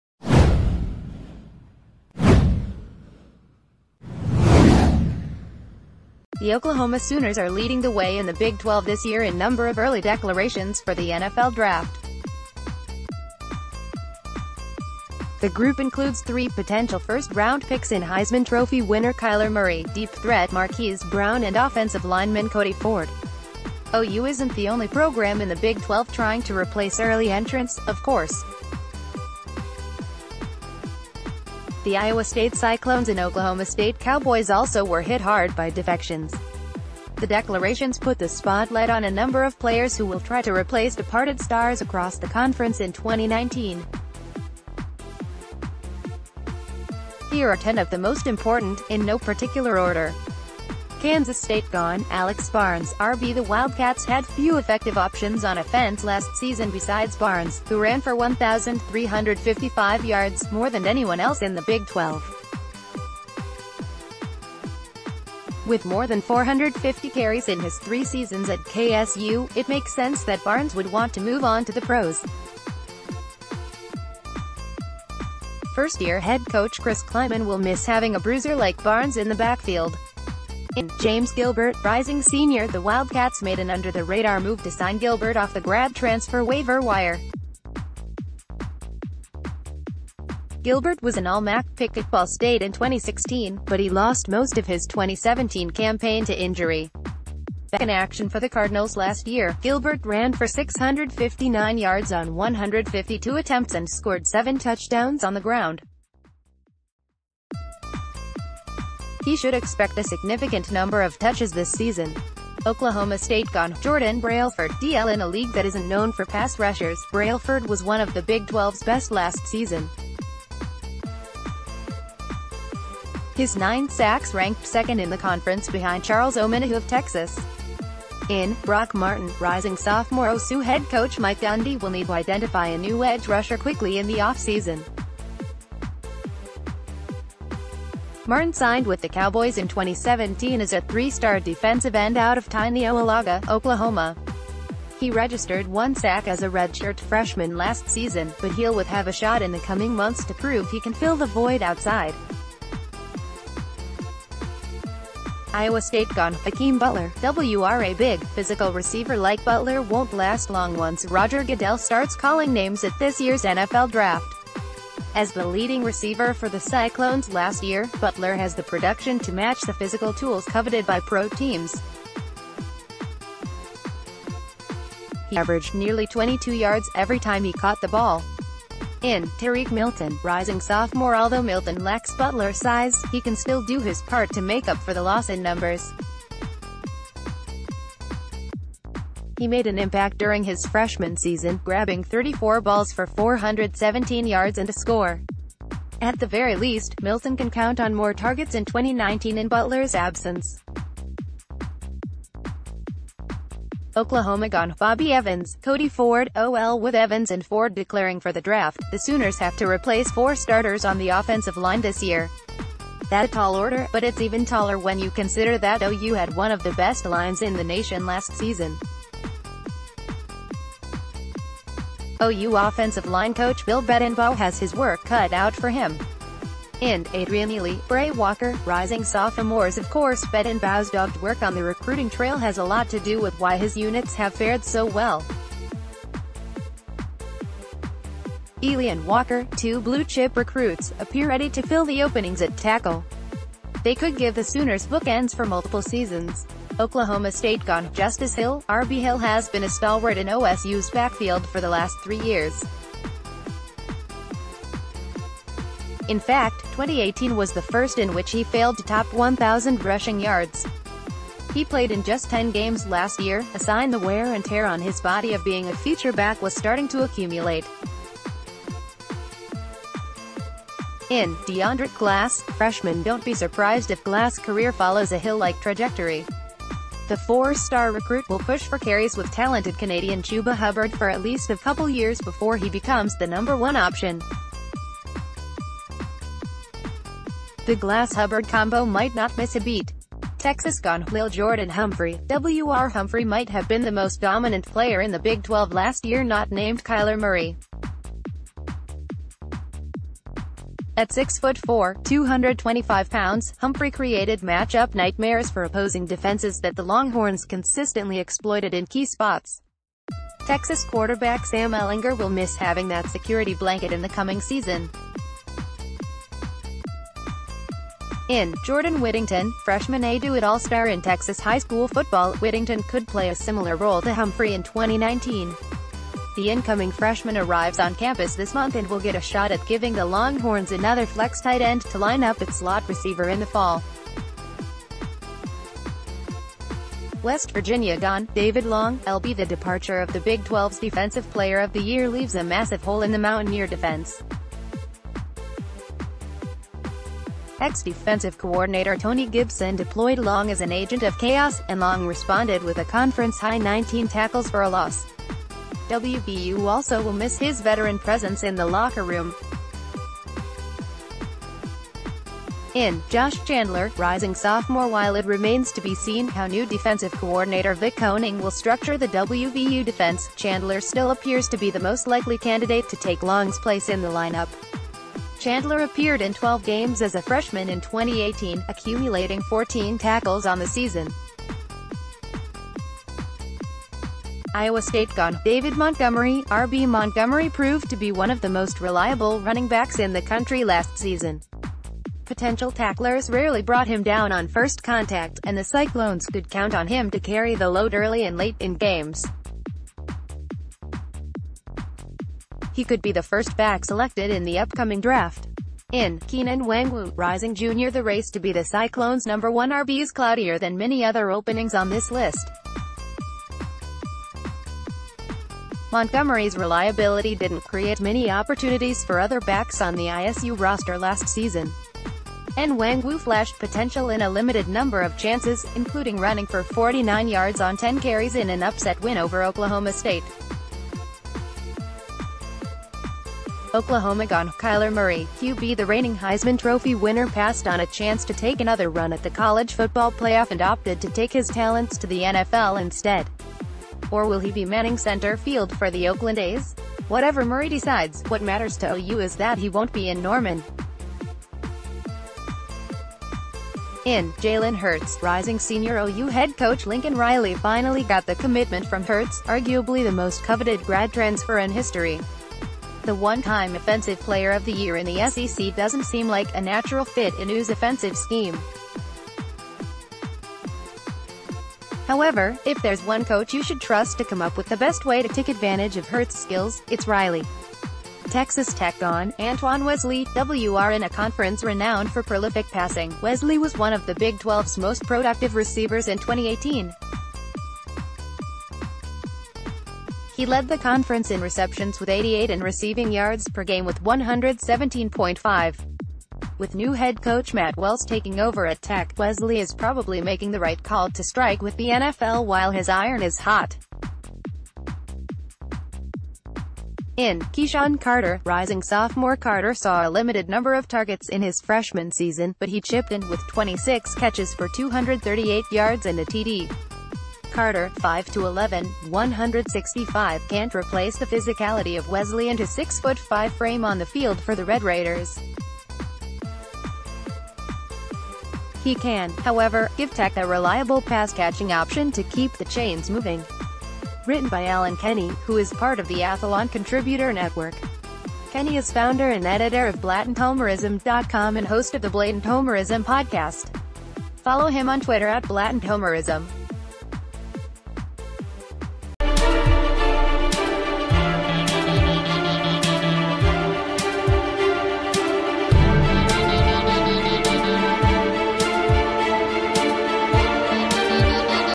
10 Big 12 Players Who Will Replace NFL Draft Early Entrants in 2019
10 Big 12 Players Who Will Replace NFL Draft Early Entrants in 2019
10 Big 12 Players Who Will Replace NFL Draft Early Entrants in 2019

The Oklahoma Sooners are leading the way in the Big 12 this year in number of early declarations for the NFL draft. The group includes three potential first-round picks in Heisman Trophy winner Kyler Murray, deep threat Marquise Brown and offensive lineman Cody Ford. OU isn’t the only program in the Big 12 trying to replace early entrants, of course. The Iowa State Cyclones and Oklahoma State Cowboys also were hit hard by defections. The declarations put the spotlight on a number of players who ...